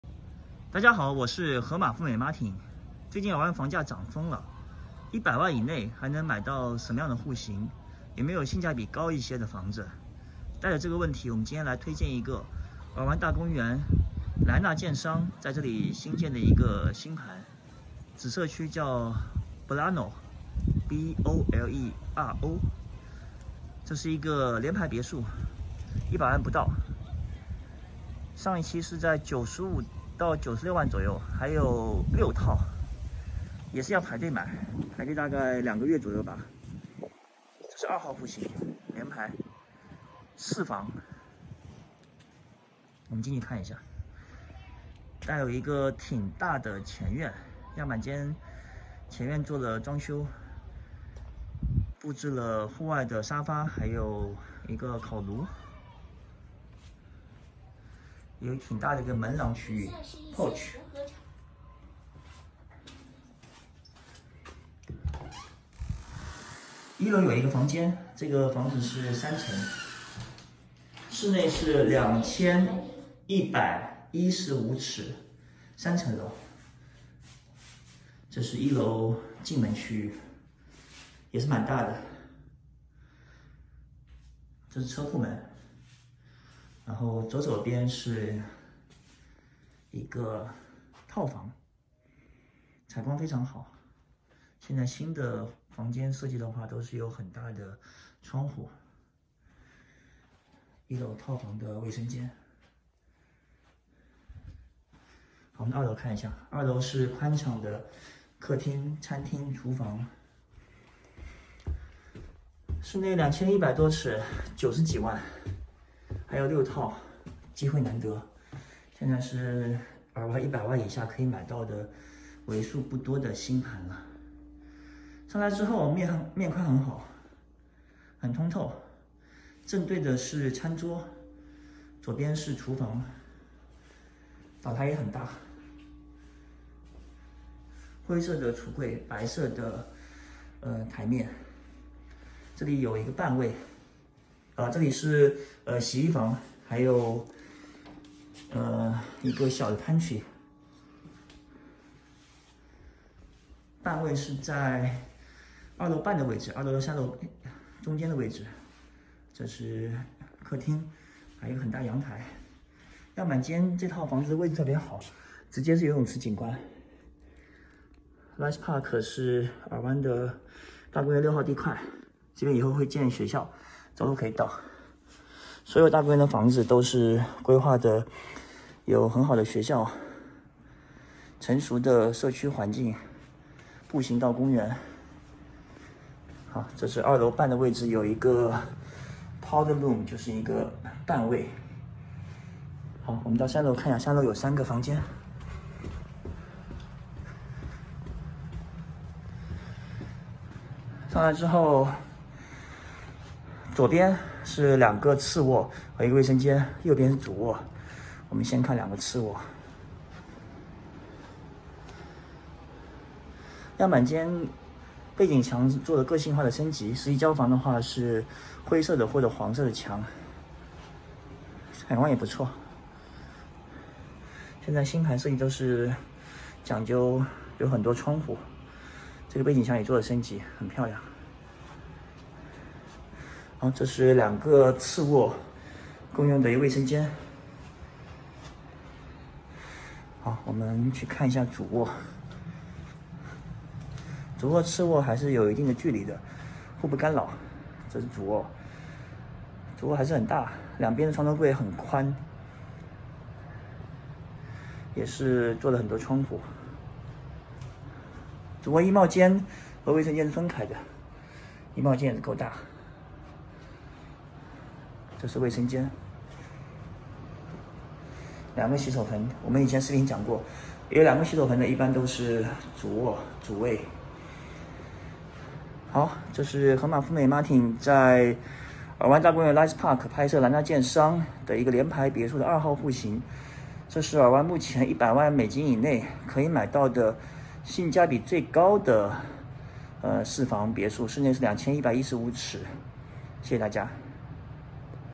在尔湾，100万内还能买什么户型？哪个新盘最具有性价比？
随着尔湾房价的快速上涨，100万以内还能买到什么户型？哪个新盘最具有性价比？今天拍摄了Lennar建商 Bolero 2号户型，售价95-96万，4房3.5卫，室内2115尺，联排别墅，3层，采光非常好，带前院，一楼一个套房和车库，二楼是客厅+餐厅+厨房，还有一个超大的阳台，三楼是1个主卧套房+2个次卧（共用1个卫生间）！Lennar建商的样板间基本上所见即所得，细节做得很好，性价比很高！错过尔湾去年的低价，不能再错过这为数不多的100万以内高性价比的新房，还剩下6套！

社区介绍：
瑞斯公园（Rise Park）是尔湾大公园6号地块（第五期），尔湾大公园是一个将休闲生活与亲近自然完美融合在一起的超级社区，瑞斯公园（Rise Park）享受的配套设施非常完善，在这里有树荫笼罩的步行和骑行小径，也有设施齐全先进的泳池温泉和聚会厅。社区周边有很多知名商圈和景点，生活十分便利。瑞斯公园隶属于尔湾联合学区，尔湾联合学区以教学质量突出，学习风气浓厚而闻名遐迩，而且享誉全球的顶尖学府加州大学尔湾分校也是近在咫尺。

公园：
Rise Park通过步道可以链接到各大公园，包括步行可达Portola高中。这里的居民可同时享受Pavilion Park，Beacon Park，Parasol Park，Cadence Park，Novel Park等众多便利设施，并可以直接进入橙县大公园，尽享欢乐亲子时光。

教育：
Rise Park隶属于尔湾联合学区，小学和初中是Cadence Park School 和Beacon Park School，高中是Portola High School。这几所学校都是新建学校，配有先进的学校设施，为孩子们提供一个更好的学习环境，而且学校就位于大公园内，孩子可以穿过大公园走路去学校。尔湾联合学区以教学质量突出，学习风气浓厚而闻名遐迩，而且享誉全球的顶尖学府加州大学尔湾分校也是近在咫尺。

交通和购物：
Rise Park位于尔湾的东侧，十分钟即可到达i-5高速，五分钟到达133收费高速。地理位置优越，生活便捷。五分钟直达最近的购物广场：Woodbury Town Center；此外，周边还有许多著名购物中心，例如美国西海岸最大的购物广场SouthCoast Plaza，以及Spectrum Center光谱购物中心、时尚岛Fashion Island等。周边旅游资源丰富，有迪士尼乐园Disneyland、Newport Beach新港海滩、Laguna Beach拉古娜海滩、Angel's Anaheim Stadium体育馆等。华人配套有99大华超市、海底捞、眉州东坡、85°C、鲜芋仙等大型连锁餐饮超市，也有同顺居、一碗粉、蜀风传奇等深受大家喜爱的特色餐馆。